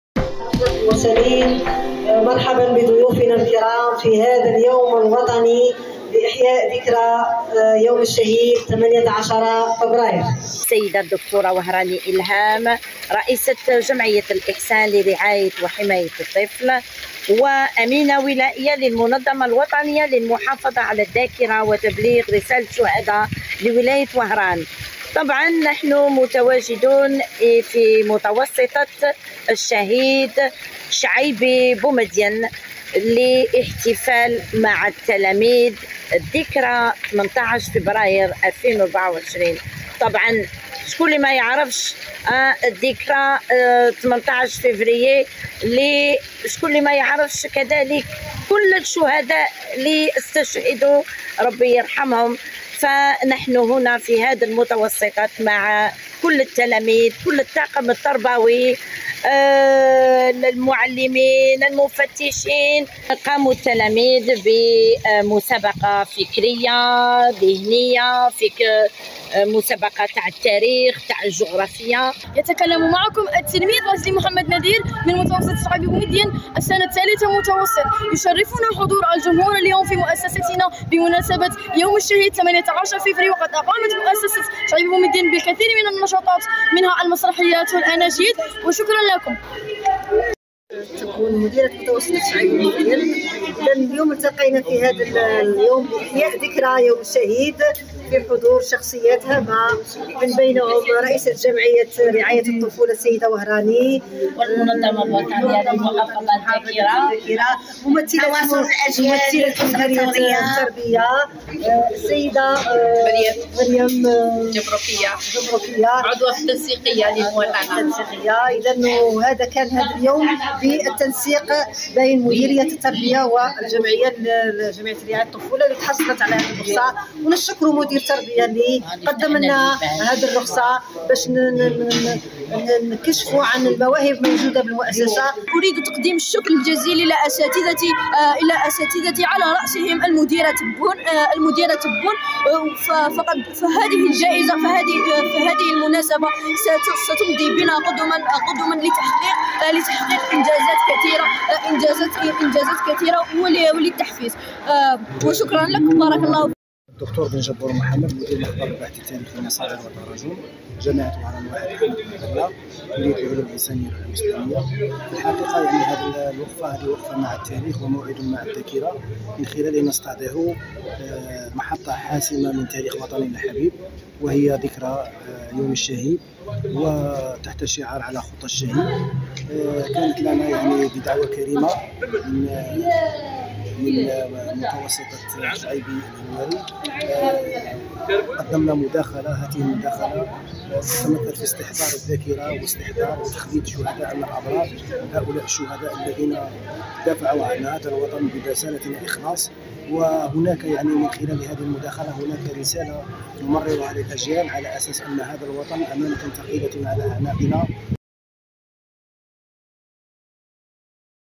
وهران: متوسطة شعيبي بومدين تقيم حفل ذكرى الشهيد بمشاركة الجمعيات الناشطة
وهران
متوسطة شعيبي بومدين تقيم حفل ذكرى الشهيد بمشاركة الجمعيات الناشطة